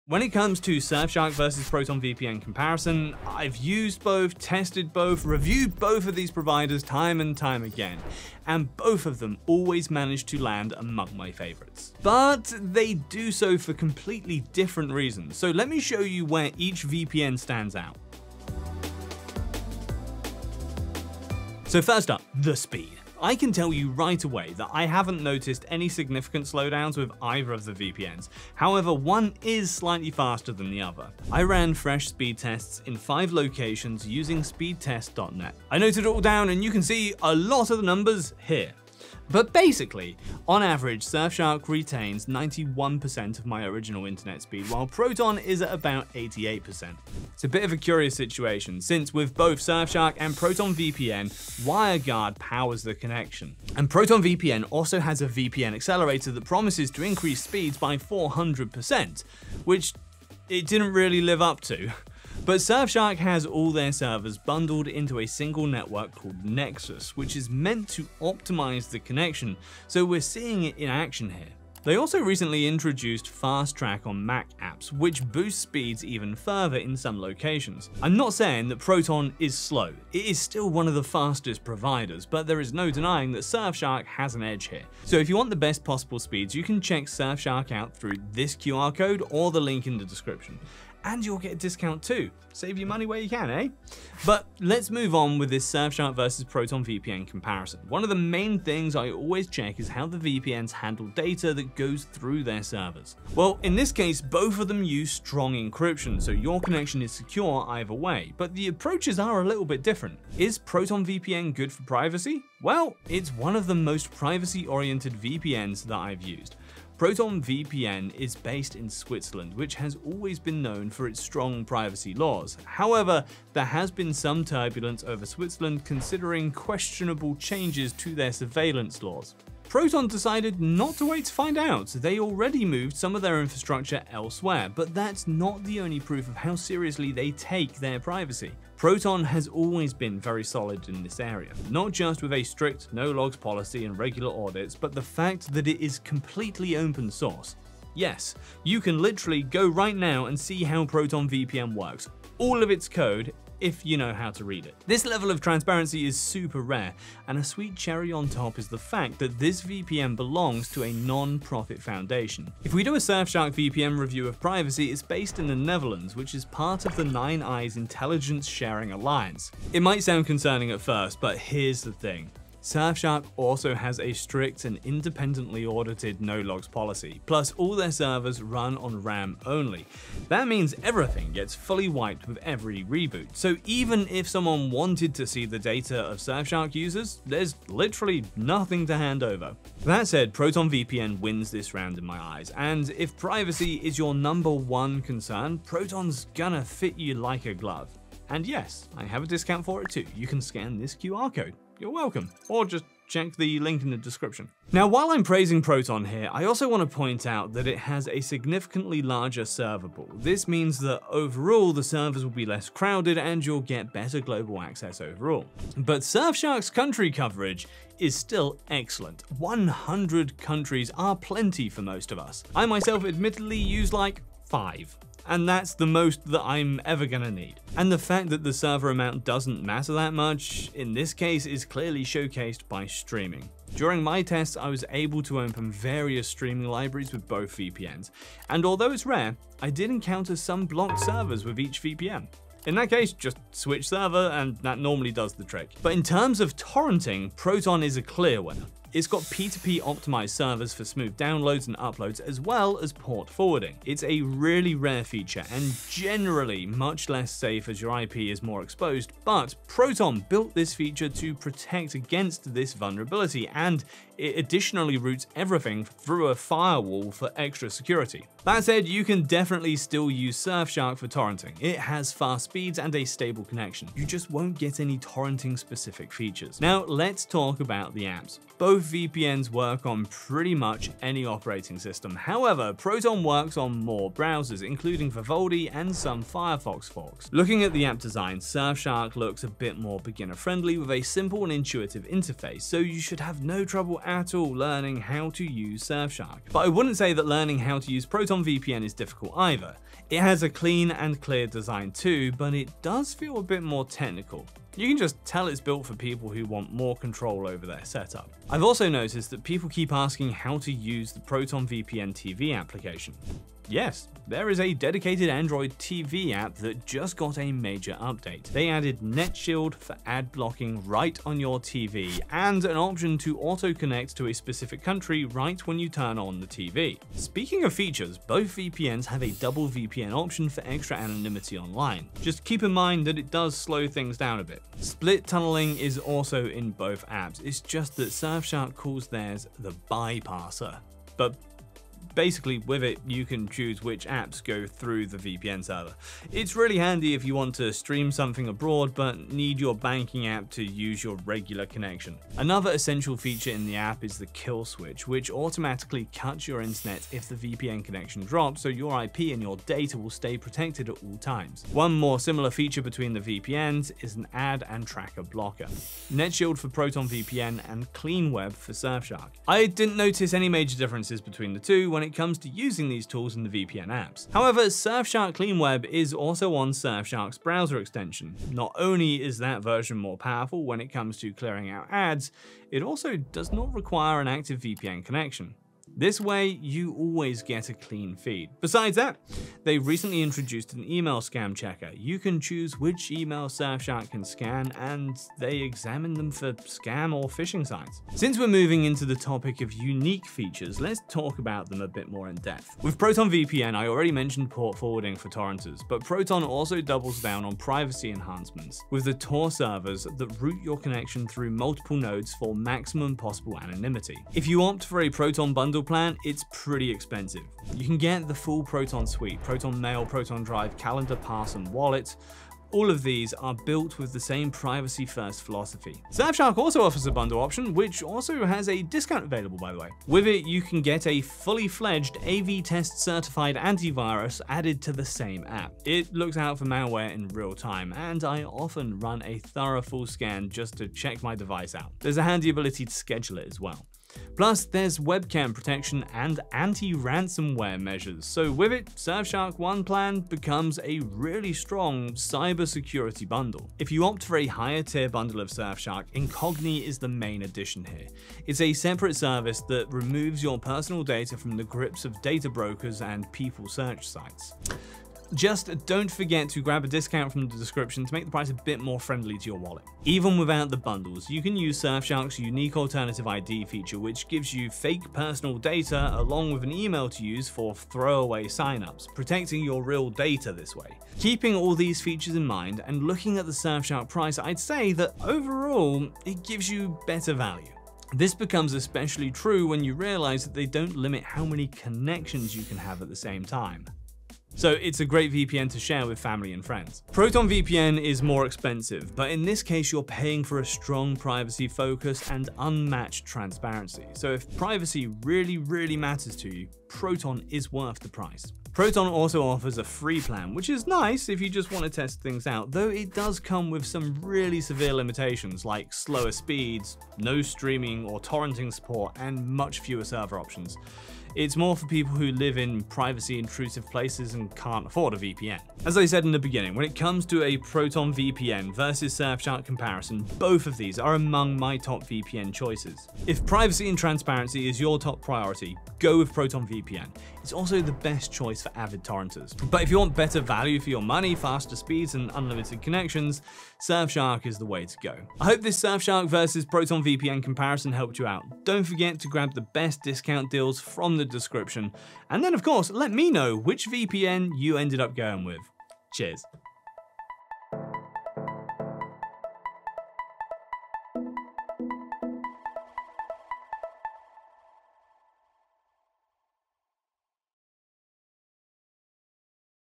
Surfshark vs Proton VPN | Which of the top VPNs is the BEST?
💥 Choose between the biggest Surfshark vs Proton VPN discounts: 💥

In a Surfshark vs Proton VPN comparison, I’ve used and tested both of these VPNs. And I like both! But today, I’m gonna show you which one is better in different areas – so you could pick one according to what you need from a VPN.

0:00 Intro
0:21 Which VPN is faster?
1:49 Is ProtonVPN better for privacy?
2:44 Surfshark privacy & RAM servers
3:26 Server networks: who wins?
3:50 Which VPN is better for streaming?
4:38 Ease of use compared
5:09 Using Proton VPN on your TV
5:28 What are the main features of each?
6:55 Pricing and bundles: which one is better?
8:46 Conclusion: which VPN should you pick?